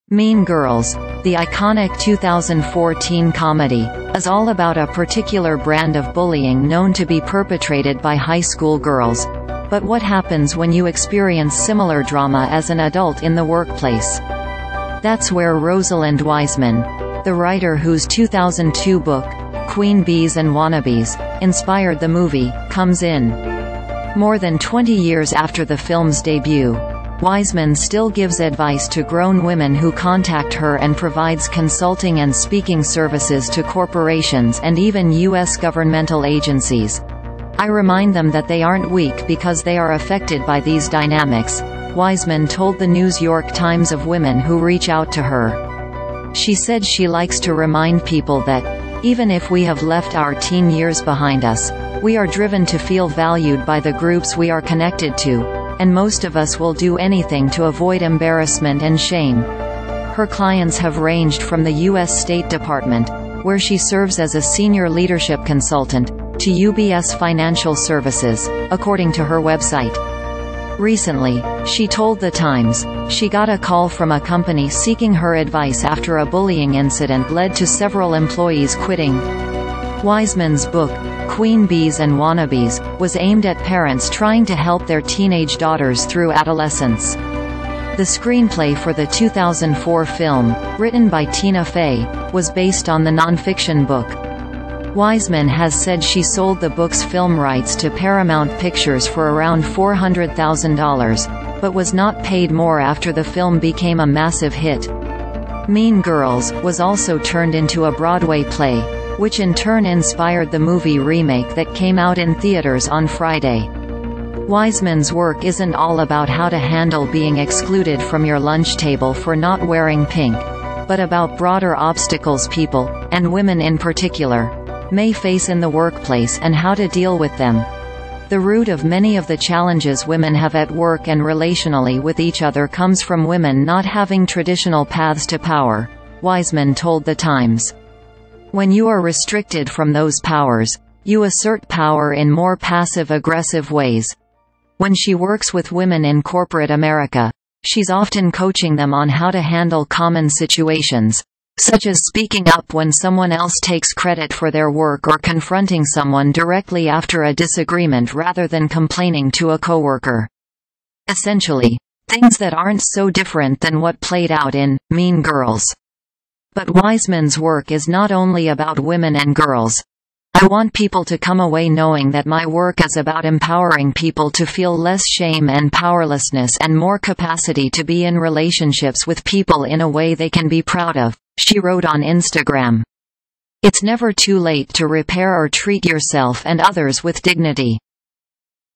The woman who inspired 'Mean Girls' now helps adults in corporate America
"Mean Girls," the iconic 2004 teen comedy, is all about a particular brand of bullying known to be perpetrated by high school girls — but what happens when you experience similar drama as an adult in the workplace? That's where Rosalind Wiseman — the writer whose 2002 book, "Queen Bees and Wannabes," inspired the movie — comes in. More than 20 years after the film's debut, Wiseman still gives advice to grown women who contact her and provides consulting and speaking services to corporations and even US governmental agencies."I remind them that they aren't weak because they are affected by these dynamics," Wiseman told The News York Times of women who reach out to her. She said she likes to remind people that "even if we have left our teen years behind us, we are driven to feel valued by the groups we are connected to, and most of us will do anything to avoid embarrassment and shame."Her clients have ranged from the US State Department, where she serves as a senior leadership consultant, to UBS Financial Services, according to her website. Recently, she told the Times, she got a call from a company seeking her advice after a bullying incident led to several employees quitting. Wiseman's book "Queen Bees and Wannabes" was aimed at parents trying to help their teenage daughters through adolescence. The screenplay for the 2004 film, written by Tina Fey, was based on the nonfiction book. Wiseman has said she sold the book's film rights to Paramount Pictures for around $400,000, but was not paid more after the film became a massive hit. "Mean Girls" was also turned into a Broadway play, which in turn inspired the movie remake that came out in theaters on Friday. Wiseman's work isn't all about how to handle being excluded from your lunch table for not wearing pink — but about broader obstacles people, and women in particular, may face in the workplace and how to deal with them."The root of many of the challenges women have at work and relationally with each other comes from women not having traditional paths to power," Wiseman told the Times. "When you are restricted from those powers, you assert power in more passive-aggressive ways."When she works with women in corporate America, she's often coaching them on how to handle common situations, such as speaking up when someone else takes credit for their work or confronting someone directly after a disagreement rather than complaining to a coworker. Essentially, things that aren't so different than what played out in "Mean Girls." But Wiseman's work is not only about women and girls."I want people to come away knowing that my work is about empowering people to feel less shame and powerlessness and more capacity to be in relationships with people in a way they can be proud of," she wrote on Instagram. "It's never too late to repair or treat yourself and others with dignity!"